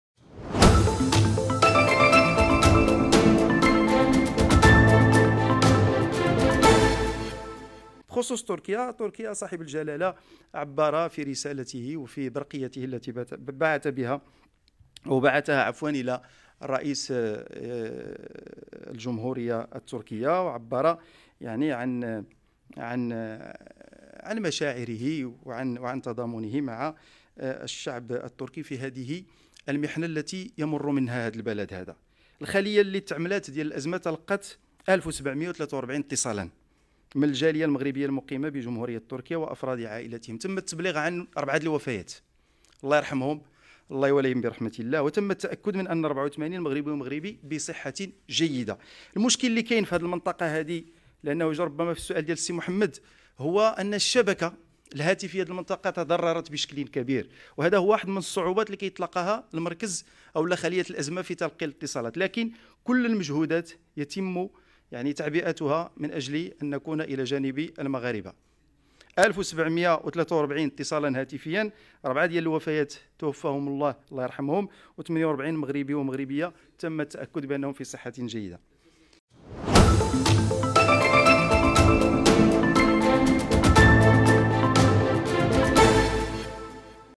تعرف على عدد الموتى المغاربة في زلزال تركيا على لسان الوزير بايتاس
في هذا العدد ستعرف على عدد الموتى المغاربة في زلزال تركيا على لسان الوزير بايتاس